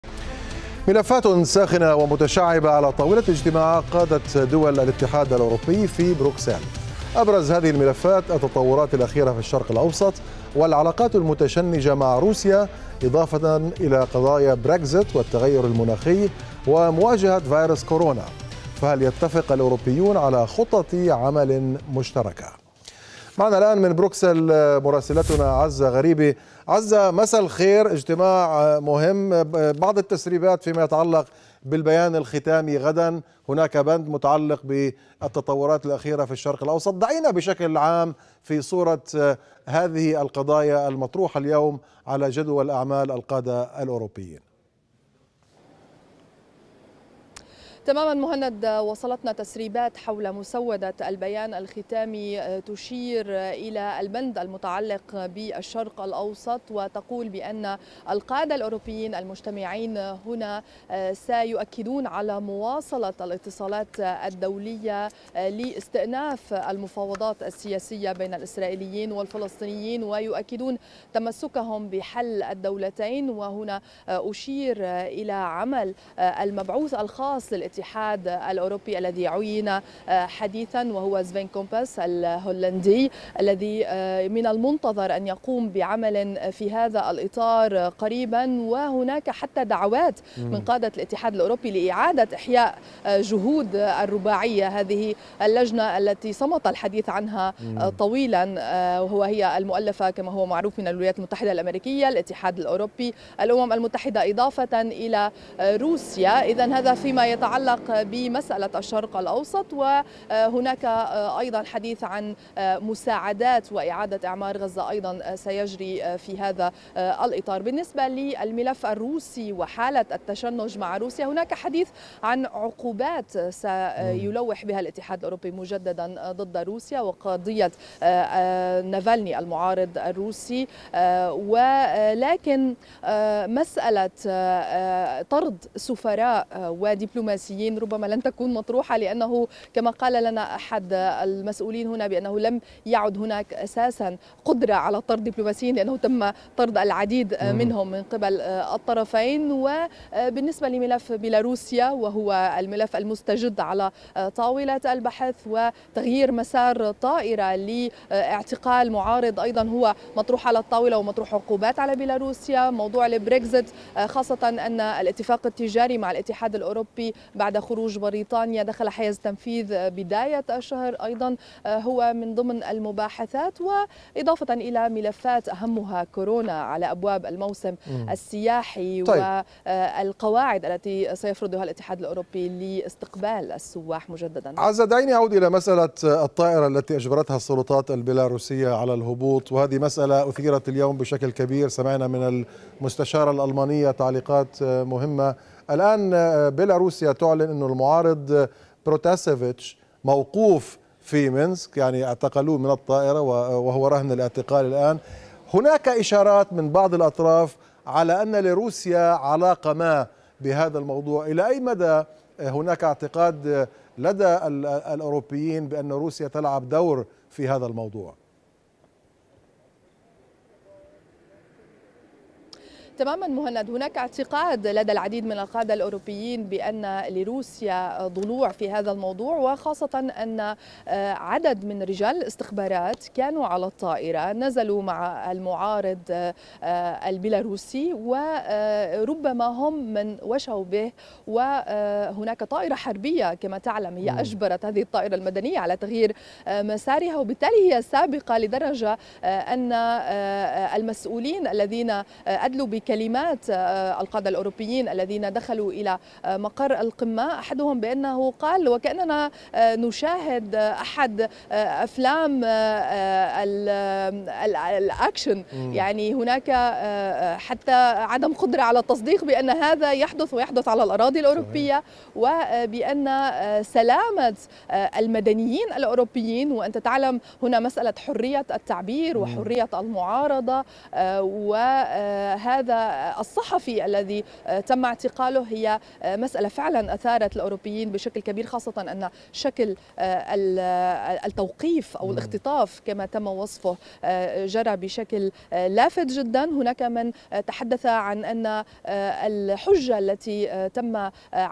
قمة الاتحاد الأوروبي.. الخلاف مع روسيا أول الملفات
ملفات ساخنة ومتشعبة على طاولة اجتماعات قادة دول الاتحاد الأوروبي في بروكسل.

أبرز هذه الملفات التطورات الأخيرة في الشرق الأوسط، والعلاقات المتشنجة مع روسيا، إضافة الى قضايا بريكسيت والتغير المناخي ومواجهة فيروس كورونا. فهل يتفق الأوروبيون على خطط عمل مشتركة؟